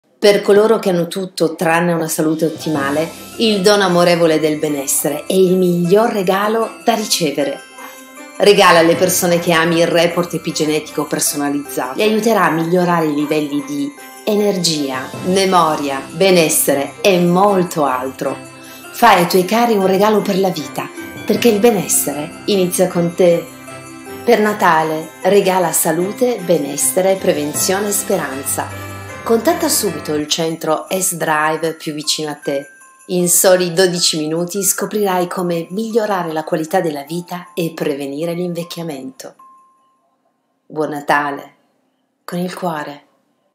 Regala bellezza benessere e prevenzione - Test Unico al mondo
Ti piacerebbe conoscere quali fattori possono condizionare la tua salute e la qualità della vita?
Quali vitamine? minerali? Amminoacidi? Acidi grassi essenziali?
Come migliorare il sistema gastrointestinale ?
Come migliorare il sistema Cardio Circolatorio?
Cosa fare per migliorare il sistema Immunitario?

Oggi puoi fare questo utilizzando una tecnologia unica al mondo
in soli 12 minuti nel centro più vicino a te.

Regala e regalati benessere e prevenzione